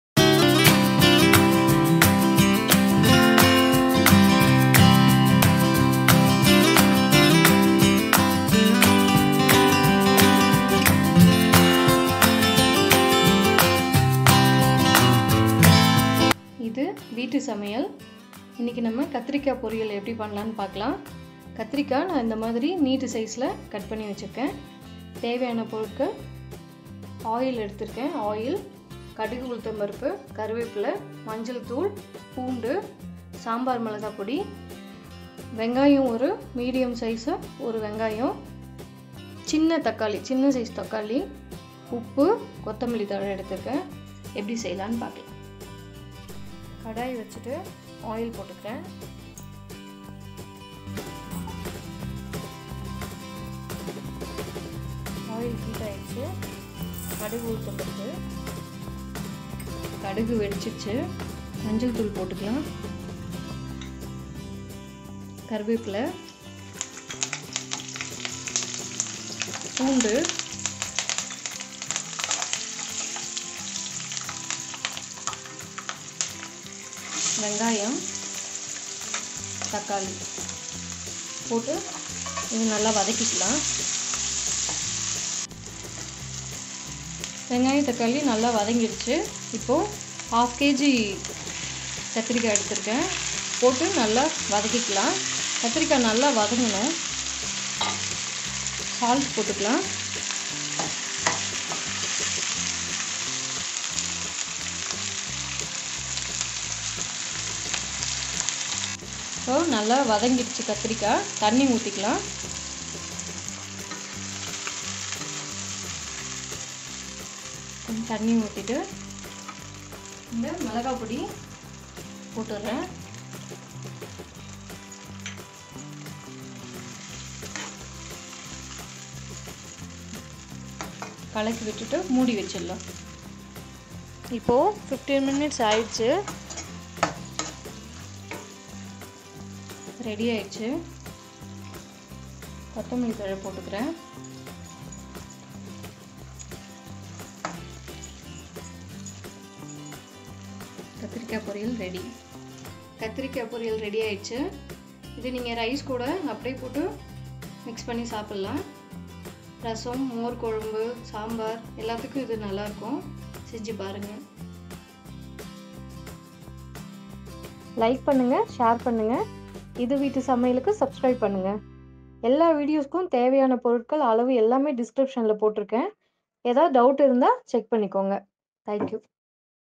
Kathirikkai Poriyal in Tamil | கத்திரிக்காய் பொரியல் | Brinjal poriyal | Eggplant Poriyal
Ingredient:
Brinjal - 1/2 Kg
Onion - 1 Med Size
Mustard - 1 Spoon
Urad Dal - 1 Spoon
Turmeric Powder - 1/2 Spoon
Garlic - 1 Med Size
Sambar Powder - 1.5 Spoon
Salt
Curry Leaves
Coriander Leaves